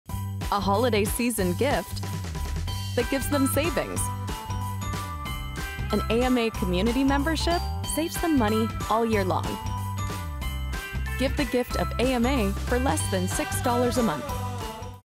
Give them the gift of Savings - AMA Community Membership
Save them $870/ year on the everyday things they already buy. Give them an AMA Community membership for less than $6/month.